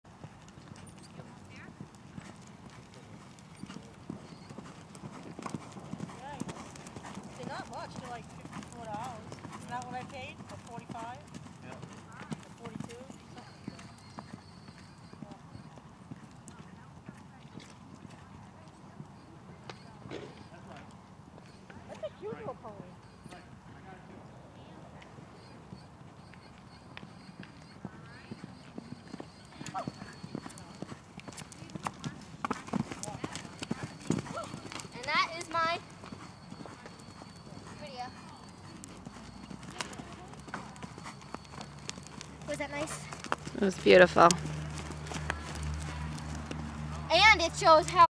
Bunny poa pony mare for sale
Haylee jumps bunny.  Bunny is a med poa pony mare she is 15yrs old and wonderful.  She is great for kids especially beginners